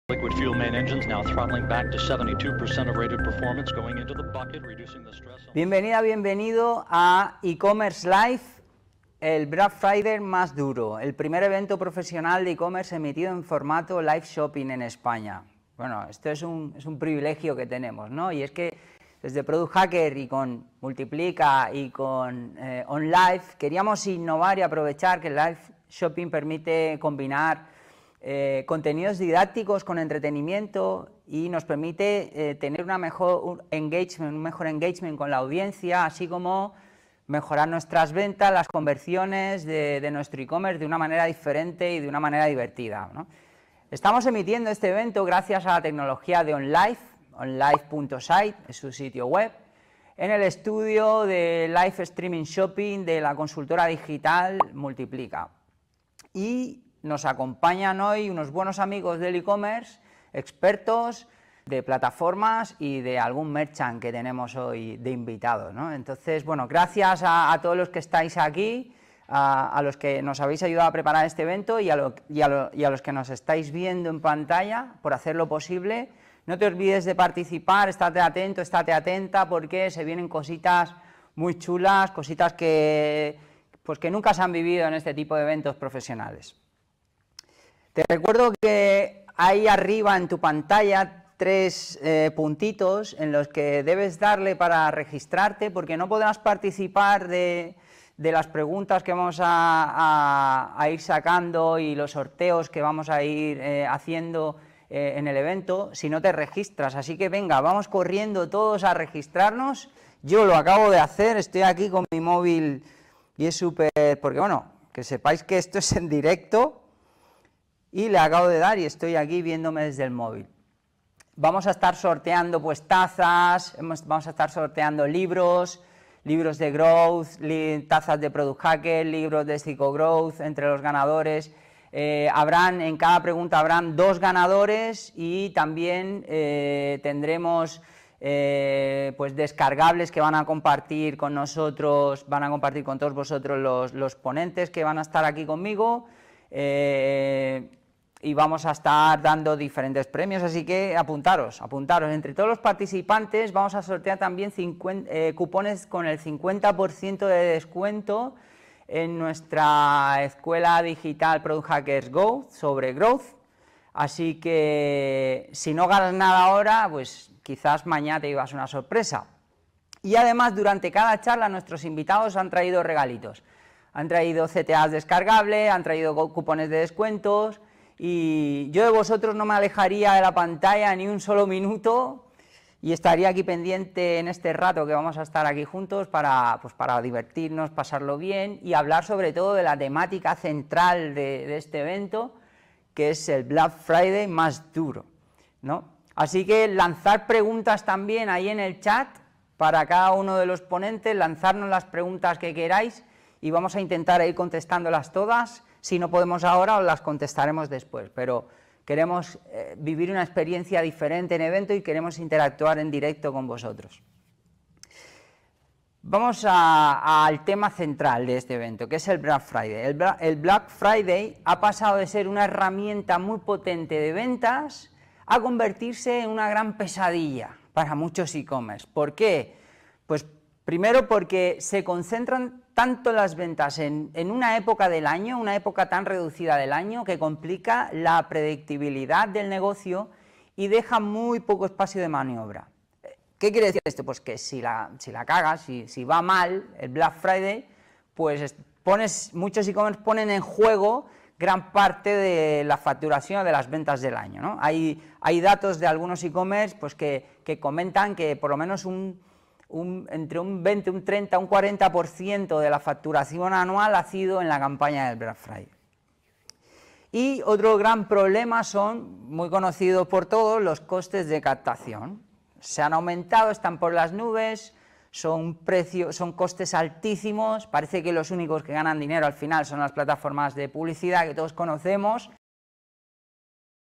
⚫ Black Friday 2022 | ¿Qué dicen los expertos?
Este Black Friday 2022 puede ser un arma de doble filo para los eCommerce: para muchos, una gran herramienta de ventas; para otros, una auténtica pesadilla. ¿Merece la pena jugarse un alto volumen de las ventas en solo unos días? ¿Puede un eCommerce predecir lo que va a vender en Black Friday? ¿Cómo influye a su capacidad de acción?

Para muchos eCommerces, la facturación del Black Friday 2022 puede llegar a un 40% del total del año. A lo largo de esta serie de vídeos, diferentes expertos del sector eCommerce nos comparten sus impresiones y fórmulas para maximizar las ventas durante el próximo 25 de noviembre. Un evento realizado en formato Live Shopping

Alfredo Duro de Onlive.site nos habla del formato Live Shopping; Alejandro Ramos de Prestashop nos cuenta las fases para que el Black Friday 2022 sea exitoso; Llorençs Palomas nos descubre el potencial de un buscador inteligente para aumentar las ventas; Diego Arroyo de Laagam nos advierte de las consecuencias de depender excesivamente del Black Friday; Alejandro Ferrari de Klarna nos invita a explorar la opción del buy now pay later; y Micky Ribera de Multiplica nos revela el concepto de brandformance.